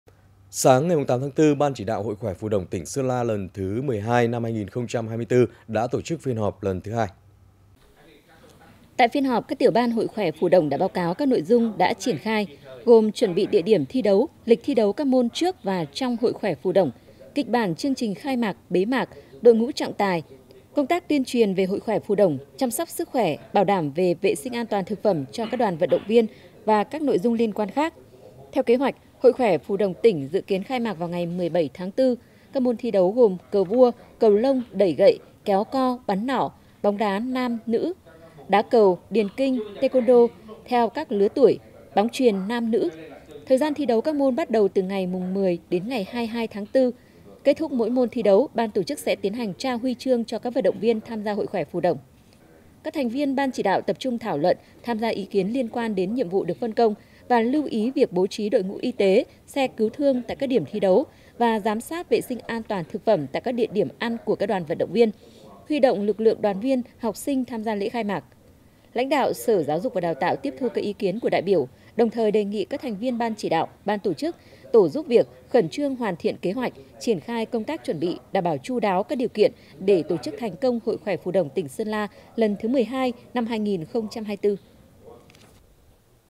Họp thống nhất nội dung, chương trình Hội khỏe Phù Đổng tỉnh lần thứ XII
Sáng ngày 8/4, Ban Chỉ đạo Hội khỏe Phù Đổng tỉnh Sơn La lần thứ XII, năm 2024, đã tổ chức Phiên họp lần thứ hai.
Theo kế hoạch, Hội khỏe Phù Đổng tỉnh dự kiến khai mạc vào ngày 17/4. Các môn thi đấu, gồm: Cờ vua, cầu lông, đẩy gậy, kéo co, bắn nỏ, bóng đá (nam, nữ), đá cầu, điền kinh, Taekwondo theo các lứa tuổi, bóng chuyền (nam, nữ). Thời gian thi đấu các môn bắt đầu từ ngày 10-22/4. Kết thúc mỗi môn thi đấu, Ban tổ chức sẽ tiến hành trao huy chương cho các vận động viên tham gia Hội Khỏe Phù Đổng.